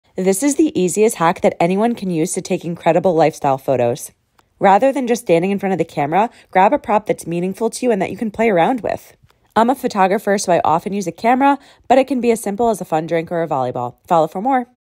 Lifestyle Photo Hack | Use Props For Photos (How To With Example Images)
This is such a fun an easy hack for anyone wanting better lifestyle images.

Props help make your photo look more realistic, and also give your model something to do and play around with.

This always results in more natural looking and fun shots.